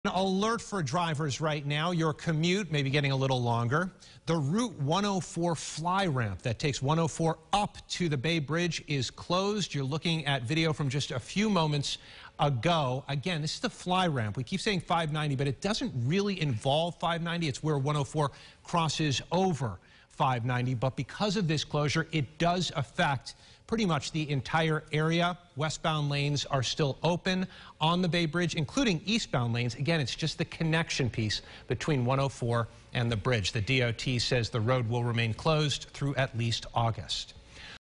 Rt. 104 flyover ramp leading to Bay Bridge closed
An eastbound flyover ramp carrying Route 104 over 590 and leading to the Bay Bridge is closed for at least a month for repairs.